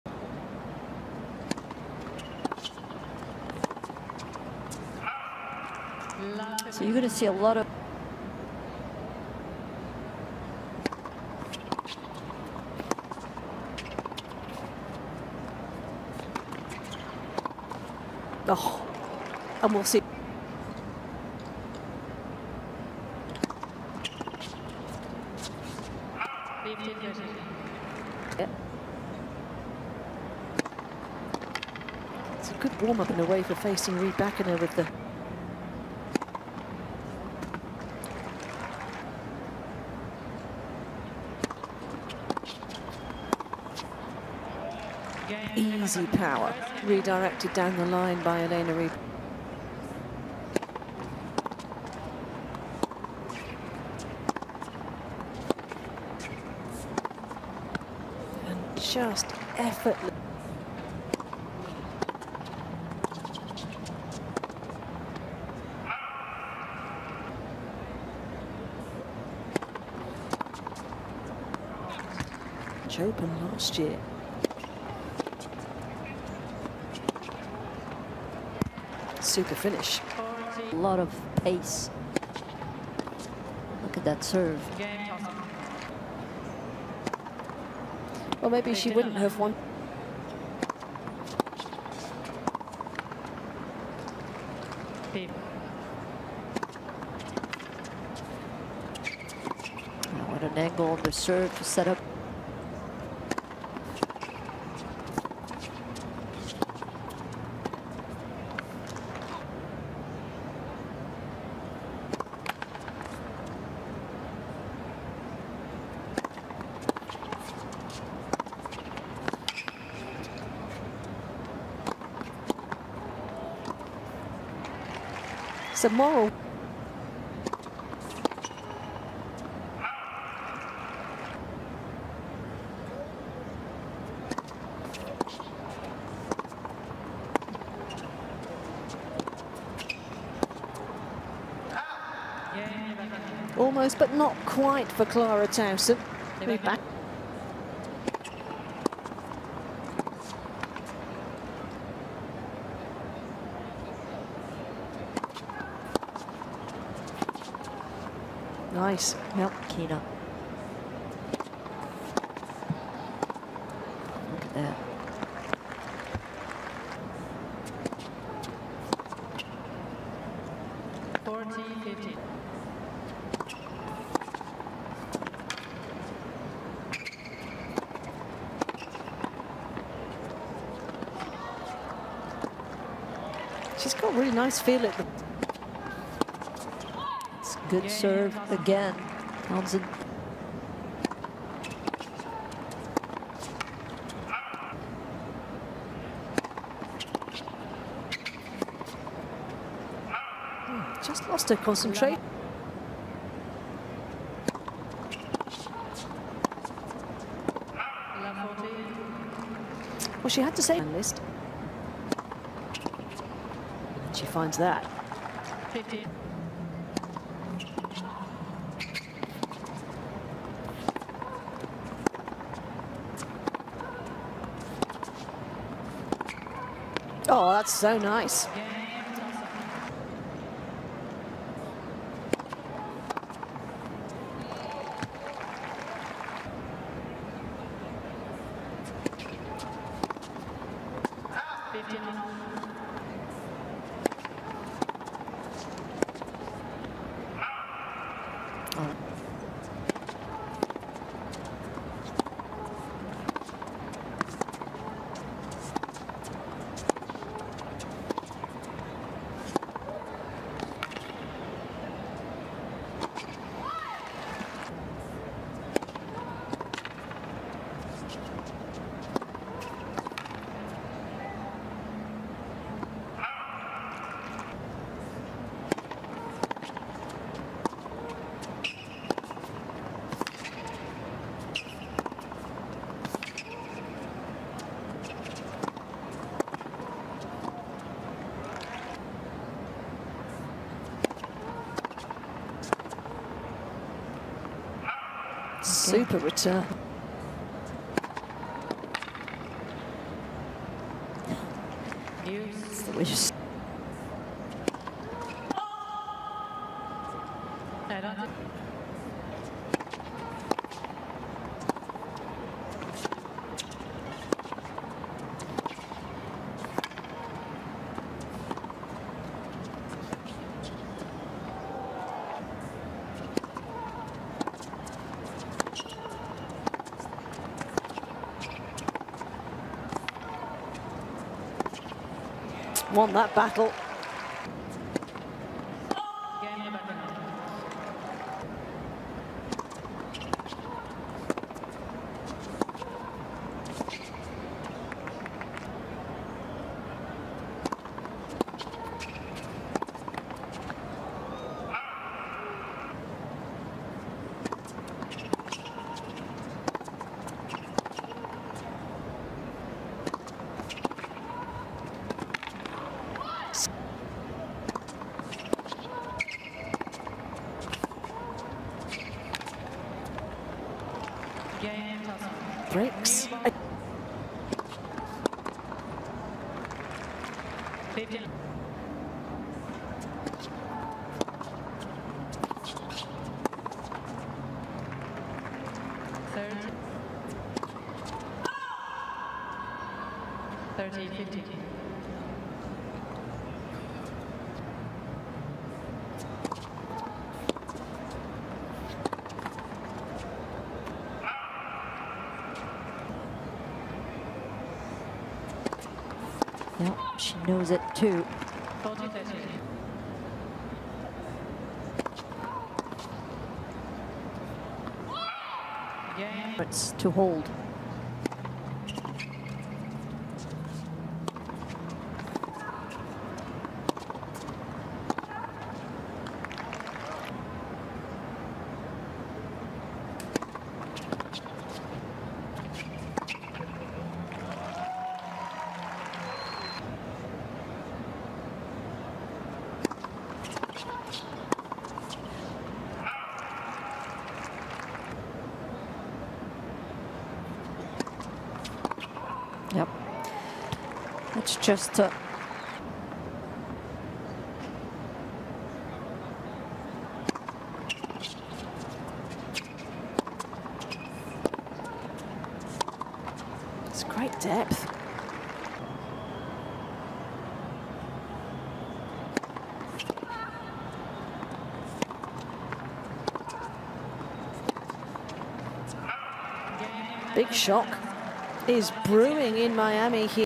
Elena Rybakina vs Clara Tauson Extended Highlights - Miami Open Tennis 2024 Round 2 Set 1
WTA Miami Open 2024 Extended Highlights Round 2 MATCH - Rybakina vs Tauson Set 1